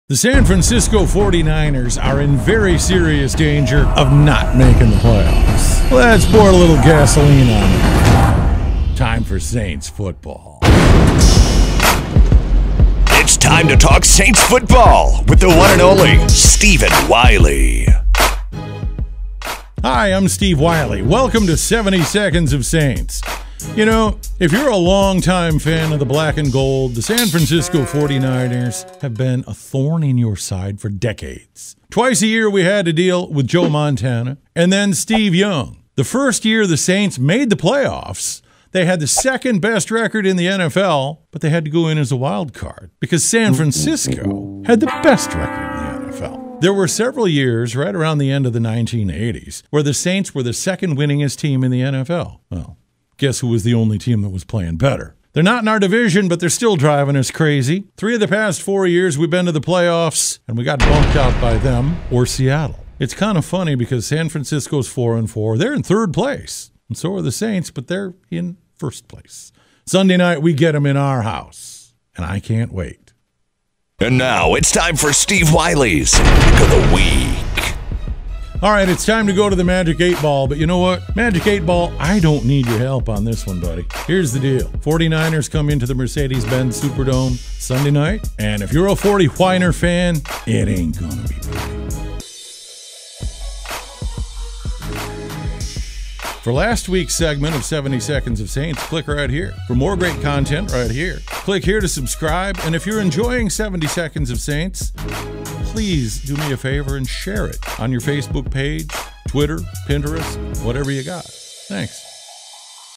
Wk. 10 49ers vs Saints: Payback Time - 70 Seconds of Saints
Despite having the same record as the NFC South 1st place Saints, the 49ers are in danger of missing out on the playoffs. Steve Wiley hopes the Saints can help squash the niners playoff hopes a bit more with a victory Sunday.